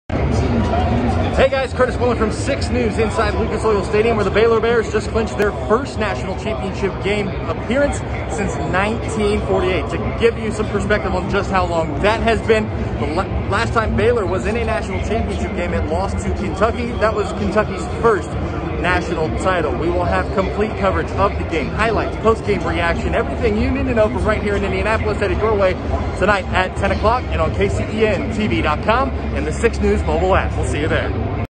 Baylor advances to first national championship games in 73 years
Sports director Kurtis Quillin is in Indianapolis where the Baylor Bears beat Houston in the Final Four.